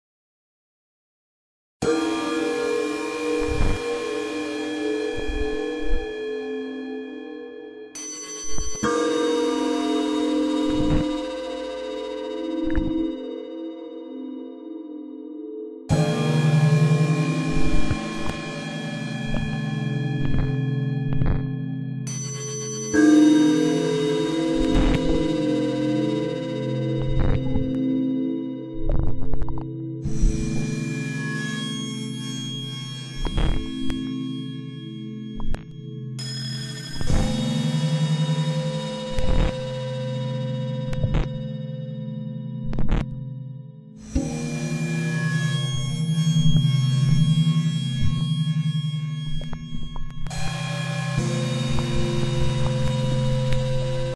The Cult Of Evil Trailer
I always wanted to make a fake B movie slasher film trailer. I thought it would be funny. It turns out it is! Enjoy!